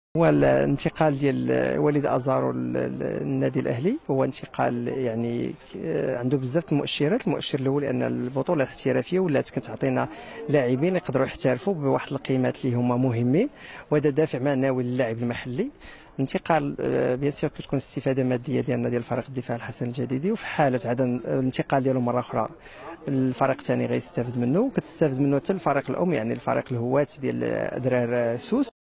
رئيس الدفاع الجديدي عبد اللطيف مقترض يتحدث عن انتقال وليد آزرو للنادي الاهلي
لمتابعتنا انضمو الينا على صفحتنا في الفايسبووك